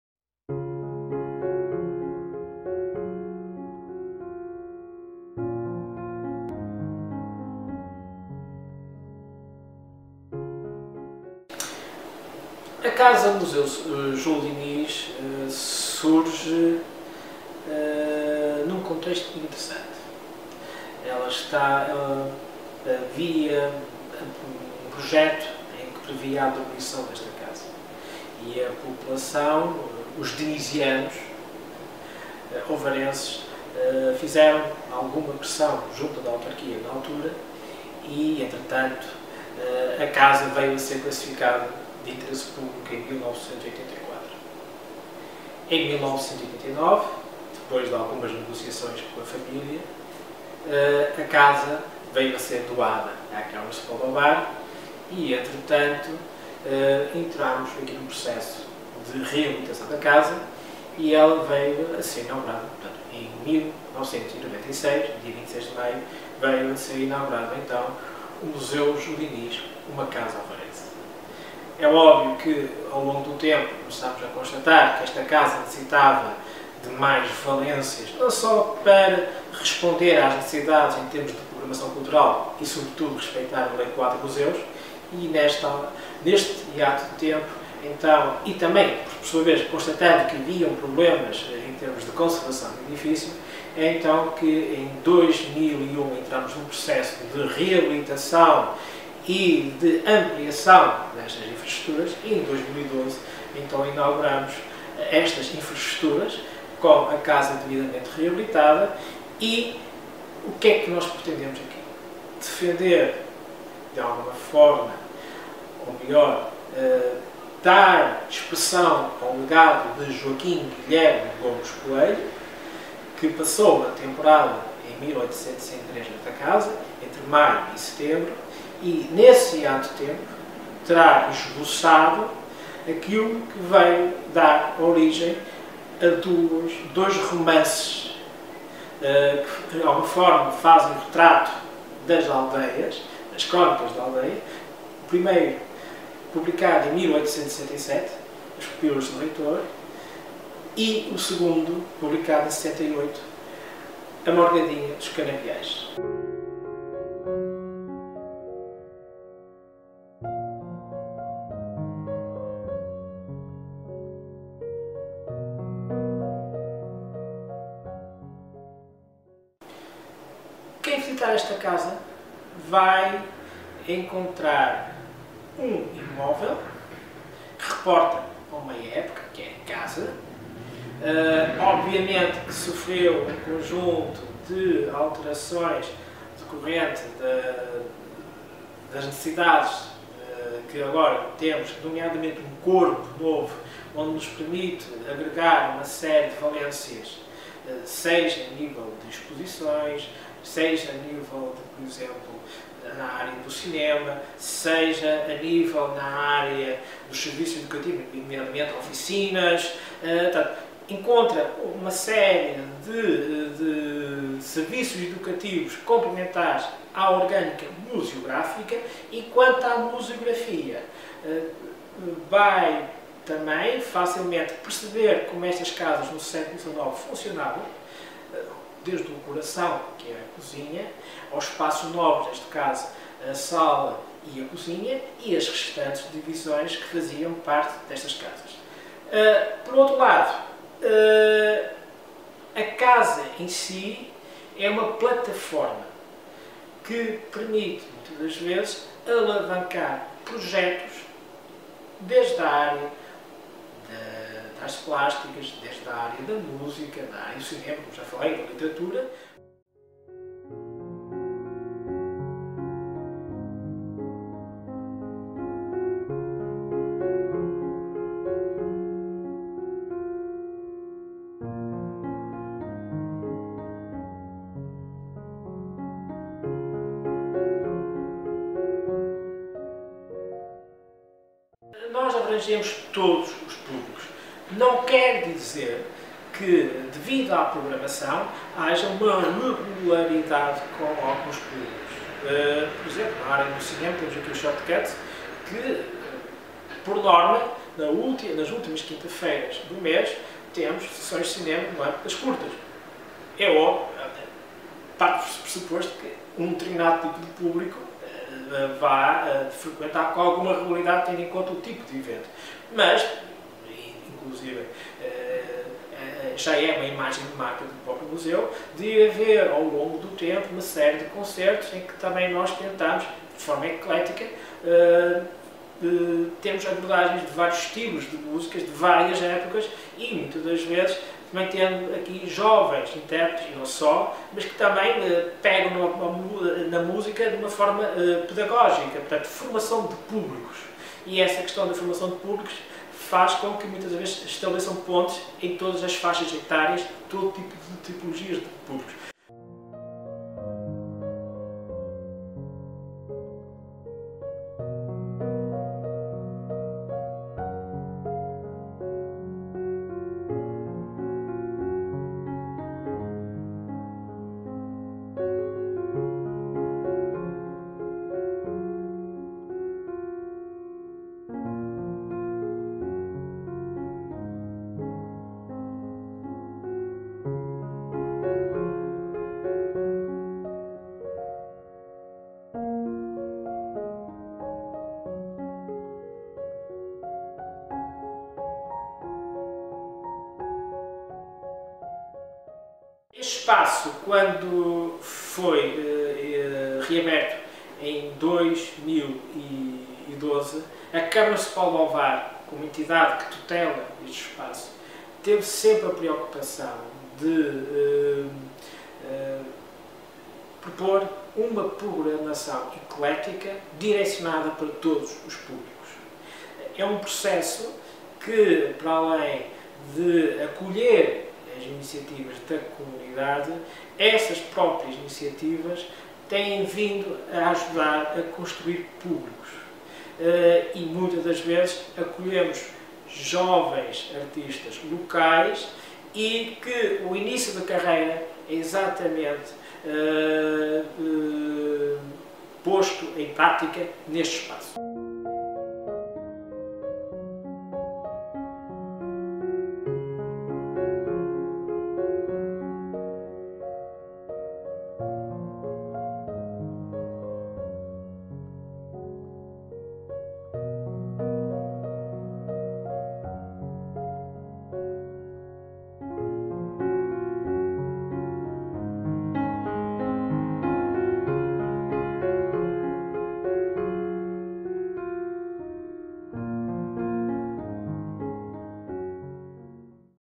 APCM _ Casa-Museu Júlio Dinis
Joaquim Guilherme Gomes Coelho, conhecido pelo pseudónimo literário Júlio Dinis, foi um médico e um autor clássico da literatura portuguesa. Viveu no Porto, mas por causa da tuberculose passou alguns meses na Casa dos Campos, em Ovar. Apesar da pequena estadia, foi de lá que surgiram duas das suas principais obras, As Pupilas do Senhor Reitor e A Morgadinha dos Canaviais. De modo a preservar o legado de Júlio Dinis e a típica casa vareira, surgiu, em 1996, o Museu Júlio Dinis – Uma Casa Vareira.